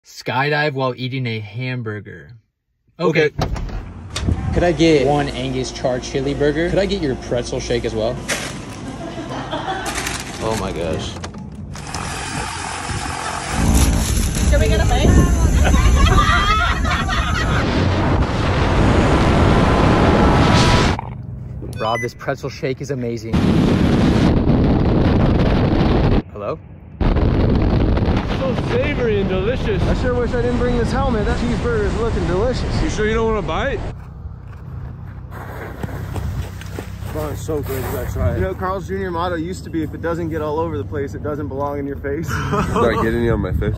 SKYDIVING WHILE EATING A CHEESEBURGER
Skydiving out of a plane while eating a cheeseburger mid flight!